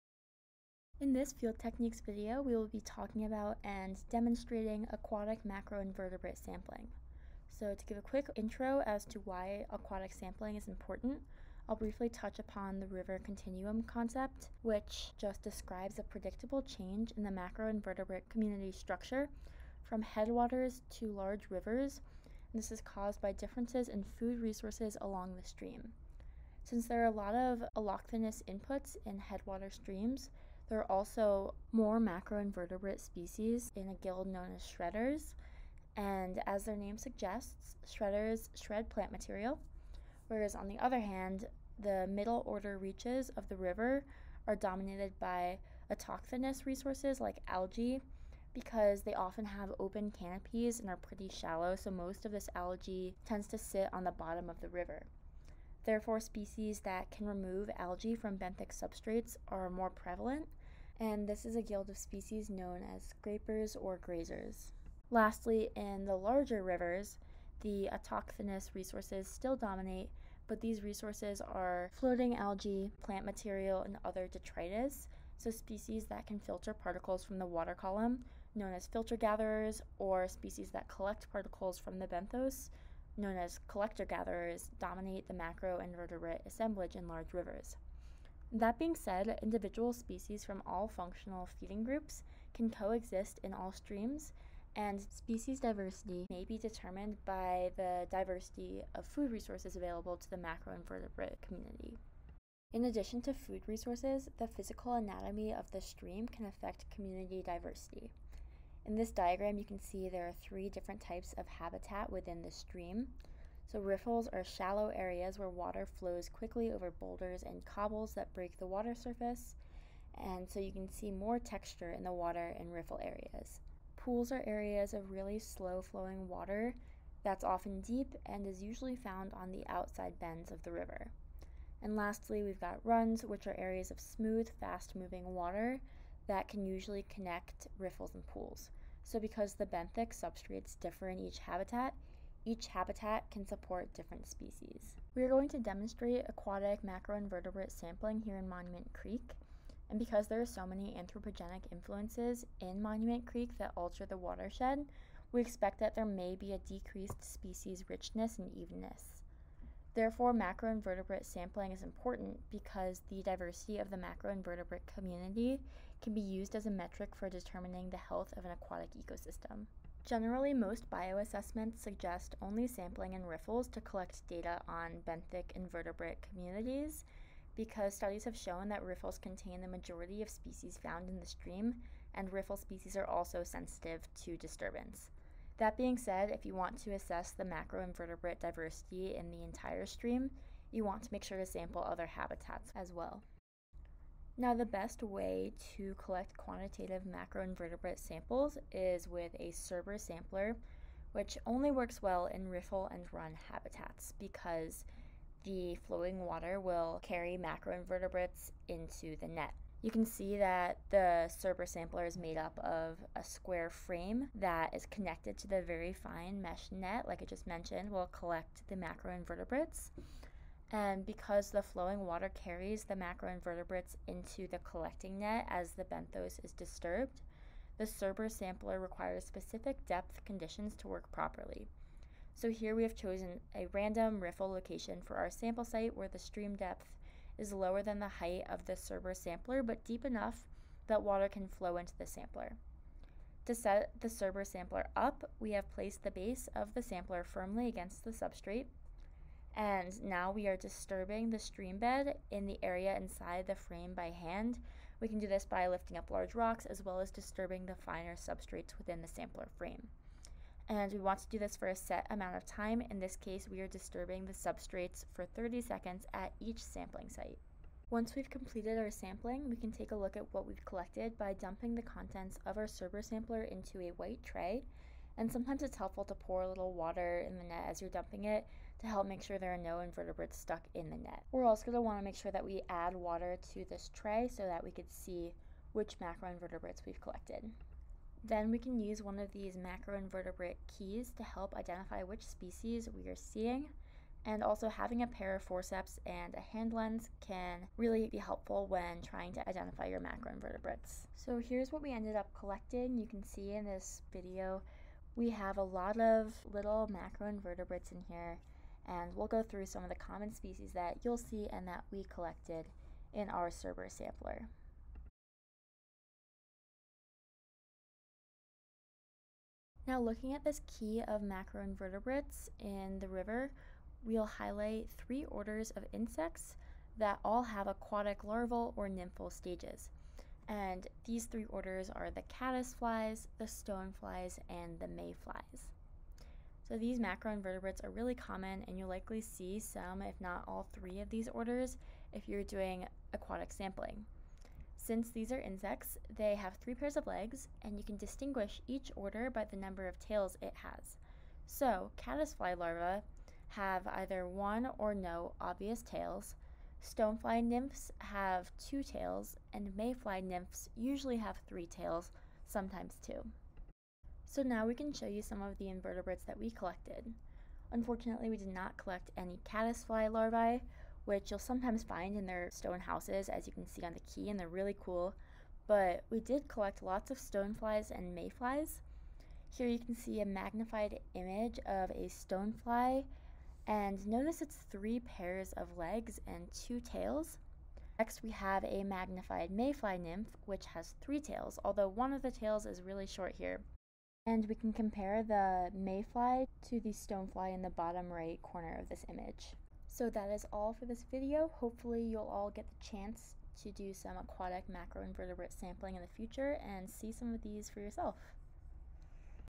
Field Techniques: Aquatic Macroinvertebrate Sampling
In this video, we discuss the River Continuum Concept and demonstrate how to sample aquatic macroinvertebrates using a Surber Sampler.